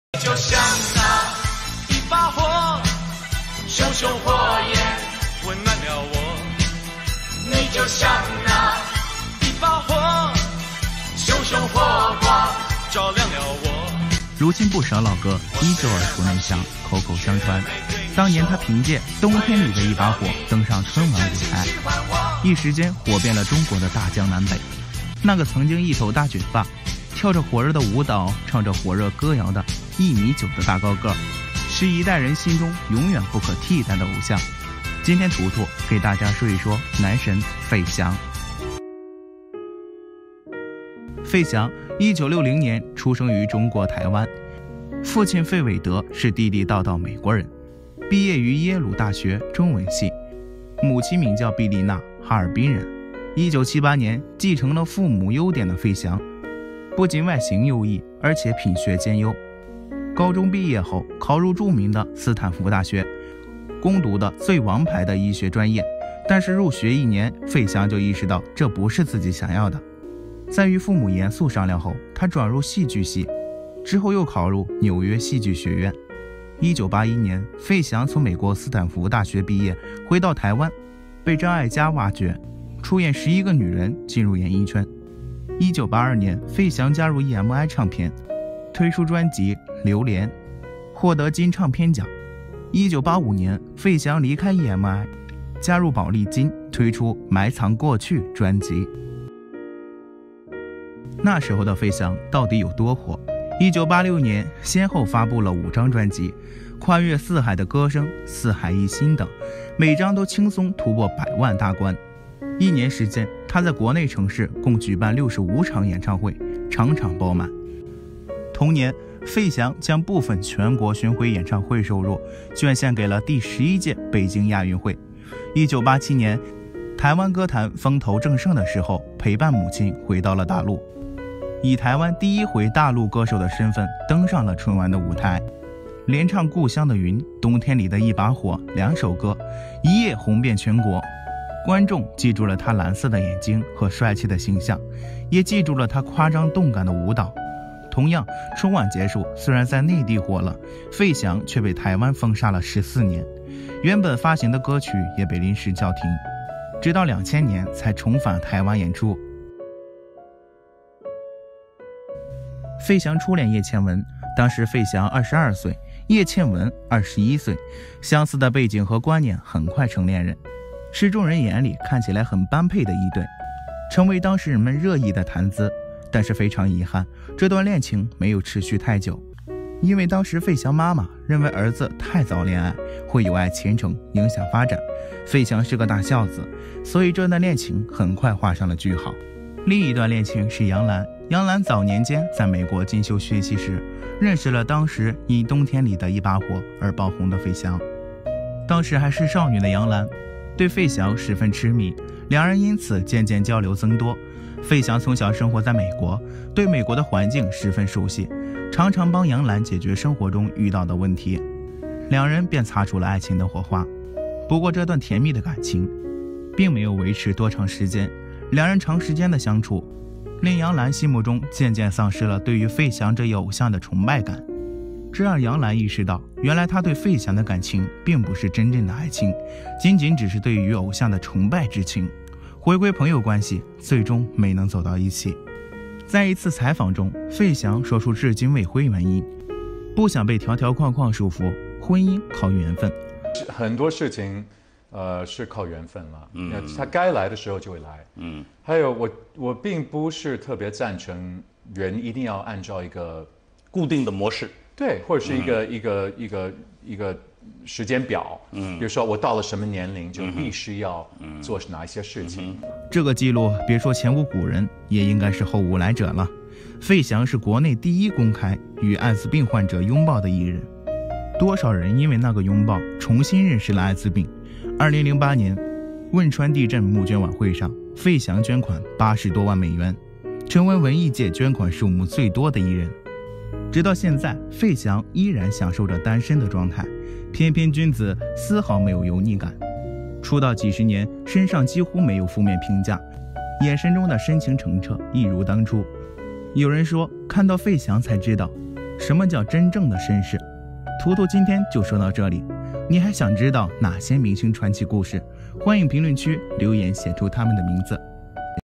费翔59岁至今未婚，叶倩文是他前女友，春晚曾红极一时~【伊力瞎掰】
费翔59岁至今未婚，叶倩文是他前女友，春晚曾红极一时
想观看更多名人的哪些事，娱乐排行榜等视频，请追踪订阅“伊力瞎掰”YouTube频道：👇🏼👇🏼 ❤️
感谢您的关注与支持❤